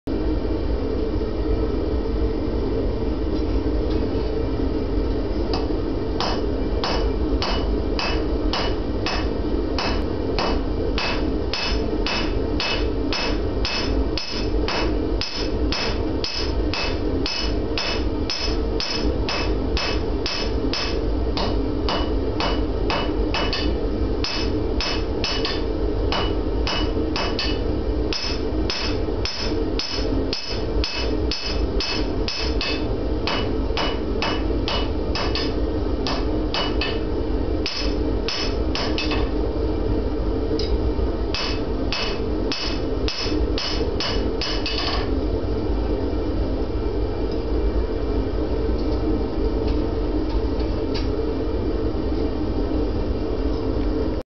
Drawing a taper
Drawing a short 2 sided taper on the end of a 1/4" x 1/2" bar.